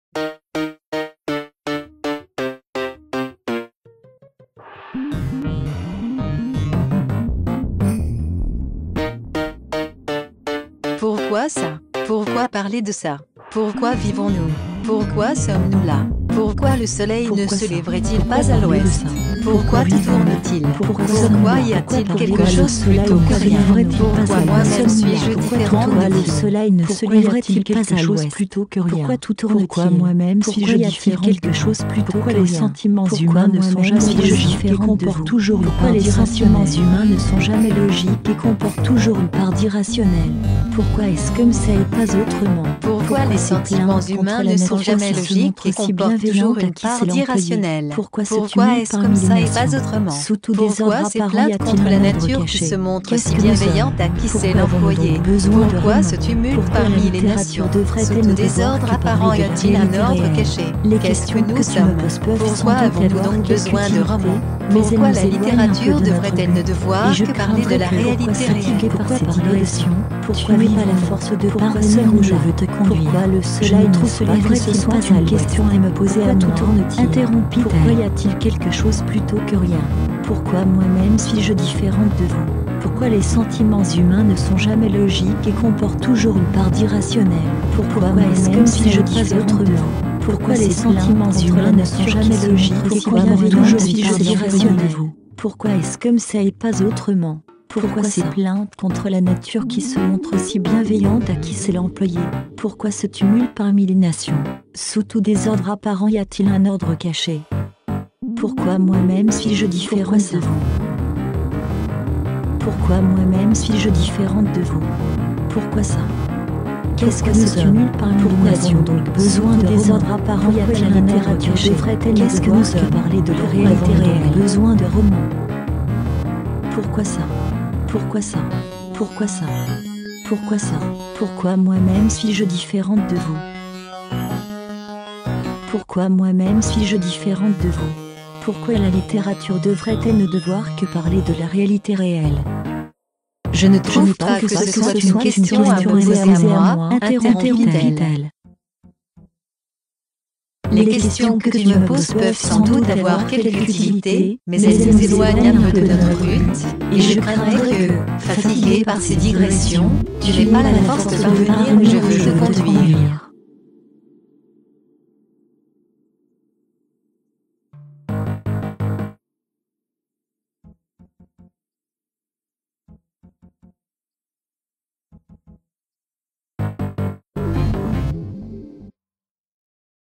Questions (Saison 1, Épisode 8)
Germaine Argencourt se pose quelques questions.